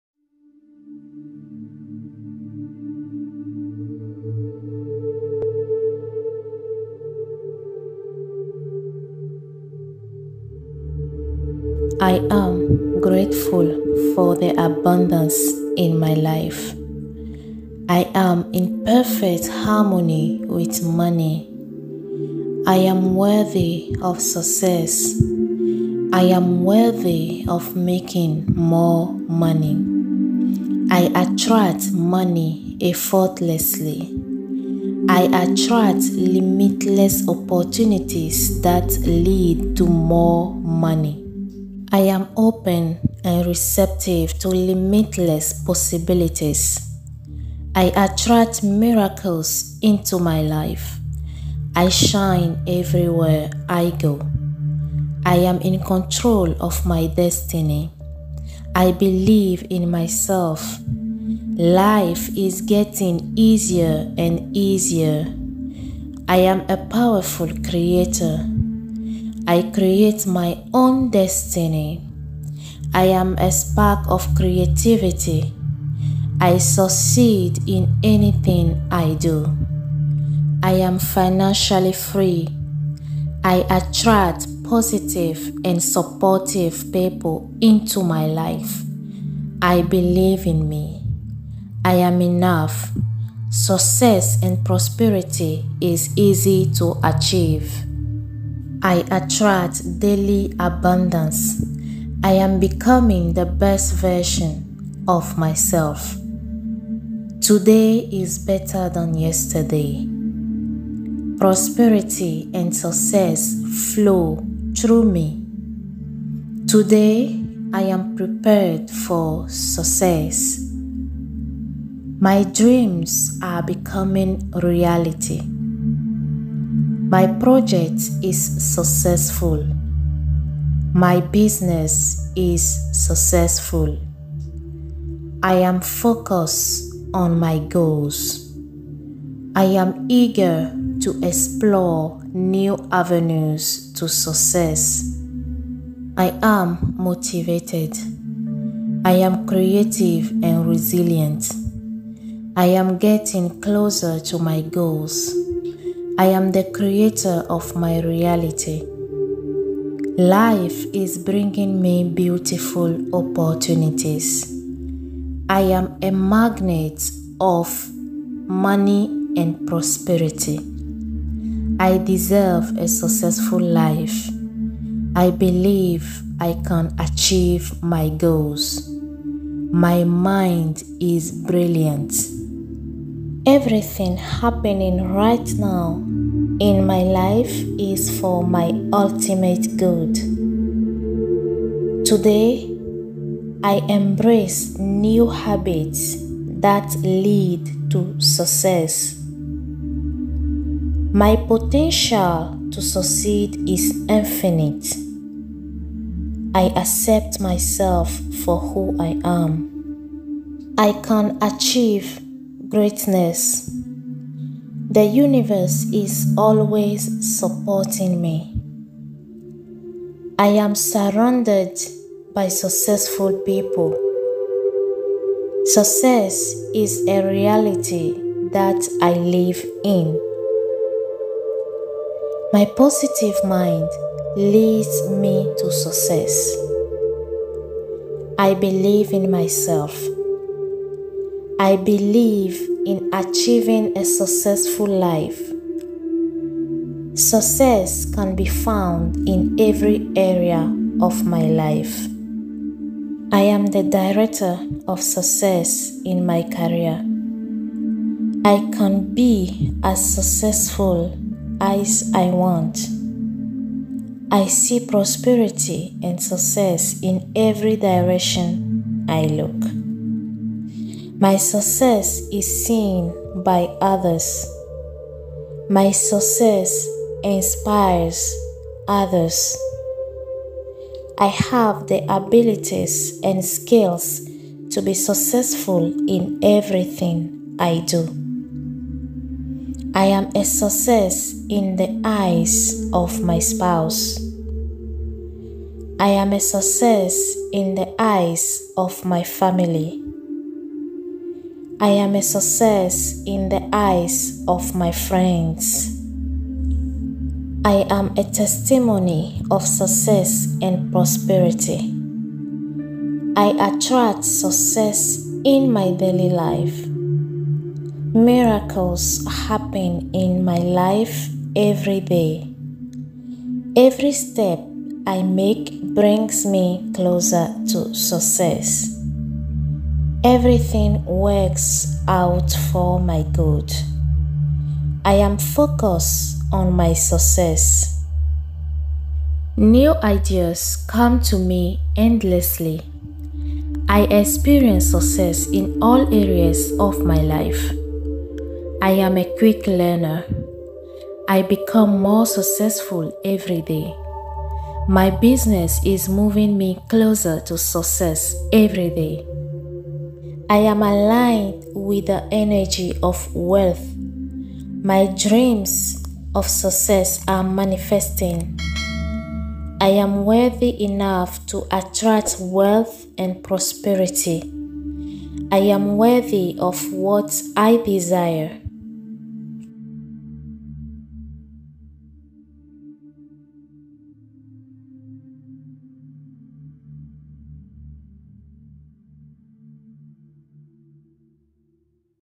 MOST POWERFUL AFFIRMATIONS FOR SUCCESS AND PROSPERITY | Listen Every Day
Use these MOST POWERFUL AFFIRMATIONS FOR SUCCESS AND PROSPERITY. Affirmations are really important to improve your mindset or you subconscious mind. If you are able to listen and repeat these affirmations for a minimum of 21 days, you will see a difference in your believe system and start attracting miracles into your life. Namaste!🙏💖